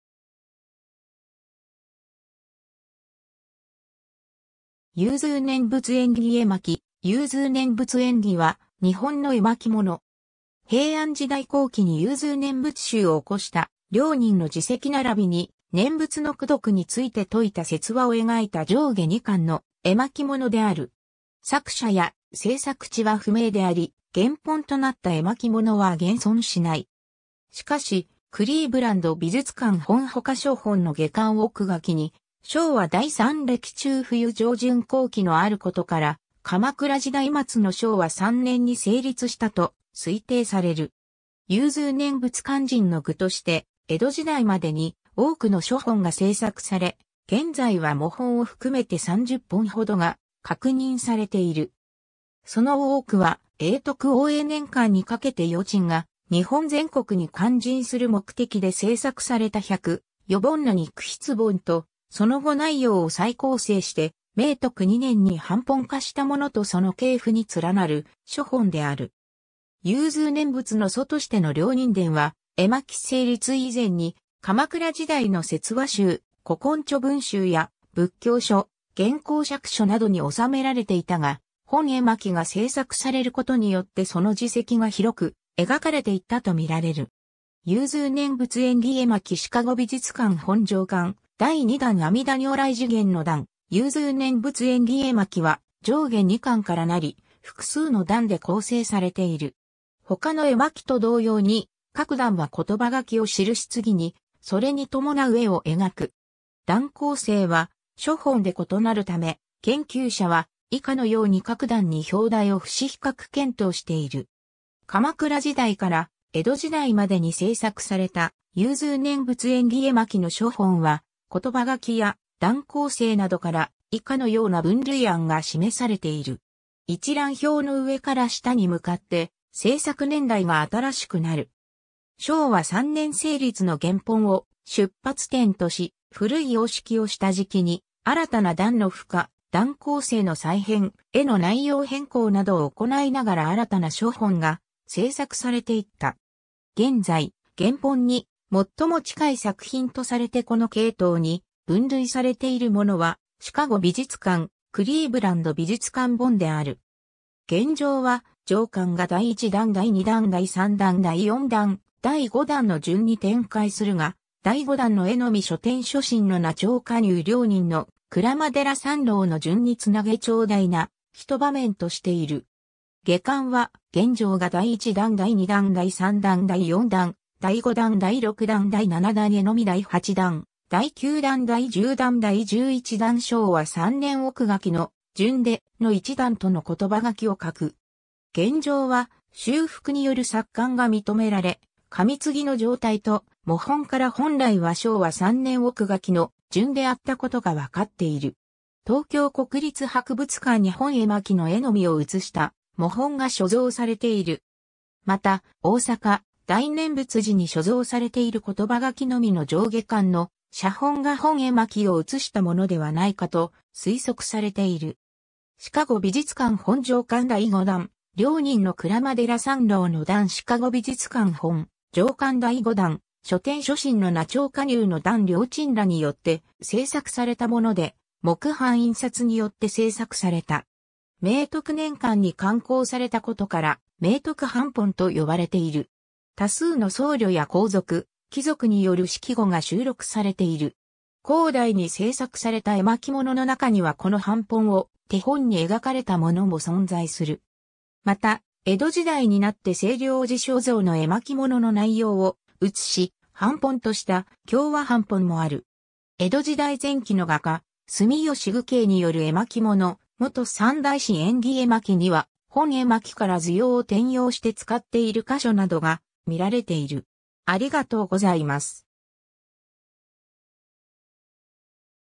融通念仏縁起絵巻
融通念仏縁起絵巻（ゆうずうねんぶつえんぎえまき）、融通念仏縁起（ゆうずうねんぶつえんぎ）は、日本の絵巻物。
 平安時代後期に融通念仏宗をおこした良忍(りょうにん)(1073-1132)の事績ならびに、念仏の功徳について説いた説話を描いた上下2巻の絵巻物である。
作者や制作地は不明であり、原本となった絵巻物は現存しない。
しかし、クリーブランド美術館本ほか諸本の下巻奥書に「正和第三歴中冬上旬候記之」あることから、鎌倉時代末の正和3年(1314年)に成立したと推定される。
 融通念仏勧進の具として江戸時代までに多くの諸本が制作され、現在は模本を含めて30本ほどが確認されている。
その多くは永徳~応永年間（1380年代~1420年代）にかけて良鎮(りょうちん)が日本全国に勧進する目的で制作された100余本の肉筆本(良鎮勧進肉筆本)とその後内容を再構成して明徳2年(1391)に版本化したもの(明徳版本)とその系譜に連なる諸本である。
 融通念仏の祖としての良忍伝は、絵巻成立以前に鎌倉時代の説話集『古今著聞集』や仏教書『元亨釈書』などに収められていたが、本絵巻が製作されることによってその事績がひろく描かれていったと見られる。
 融通念仏縁起絵巻シカゴ美術館本 上巻 第2段 阿弥陀如来示現の段 融通念仏縁起絵巻は上下2巻からなり複数の段(場面)で構成されている。
他の絵巻と同様に、各段は詞書を記し次にそれに伴う絵(画面)を描く。
段構成は諸本で異なるため、研究者は以下のように各段に表題を付し比較検討している。
 鎌倉時代から江戸時代までに制作された融通念仏縁起絵巻の諸本は、詞書や段構成などから以下のような分類案が示されている。
一覧表の上から下に向かって制作年代が新しくなる。
正和3年成立の原本(正和本、正和原本)を出発点とし、古い様式を下敷きに新たな段の付加・段構成の再編・絵(画面)の内容変更などを行ないながら新たな諸本が制作されていった。
 現在、原本に最も近い作品とされてこの系統に分類されているものは、シカゴ美術館・クリーブランド美術館本である。
現状は上巻が第1段(1)→第2段(2)→第3段(3)→第4段(4)→第5段(5)(6)の順に展開するが、第5段の絵のみ(6)諸天諸神の名帳加入→(5)良忍の鞍馬寺参籠の順に繋げ長大な一場面としている。
下巻は現状が第1段(7)→第2段(8)→第3段(9)→第4段(10)→第5段(11)→第6段(12)→第7段(14)絵のみ→第8段(15)→第9段(16)→第10段(17)→第11段(18)→正和3年奥書の順で、(13)の一段と(14)の詞書を欠く。
現状は修復による錯簡が認められ、紙継の状態と模本から本来は(16)→(18)→(17)→正和3年奥書の順であったことが分かっている。
東京国立博物館に本絵巻の絵のみを写した模本が所蔵されている　　。
また、大阪・大念仏寺に所蔵されている詞書のみの上下巻の写本(大念仏寺蔵絵詞本)が本絵巻を写したものではないかと推測されている。
 シカゴ美術館本 上巻 第5段「良忍の鞍馬寺参籠の段」 シカゴ美術館本 上巻 第5段「諸天諸神の名帳加入の段」 良鎮らによって製作されたもので、木版印刷によって製作された。
明徳年間に刊行されたことから「明徳版本」と呼ばれている。
多数の僧侶や皇族・貴族による識語が収録されている。
後代に製作された絵巻物の中にはこの版本を手本に描かれたものも存在する。
また、江戸時代になって清涼寺所蔵の絵巻物の内容を写し、版本とした「享和版本」（享和元年・1801年）もある。
 江戸時代前期の画家・住吉具慶（すみよしぐけい）による絵巻物『元三大師縁起絵巻』（1679年）には、本絵巻から図様を転用して使っている箇所などが見られている。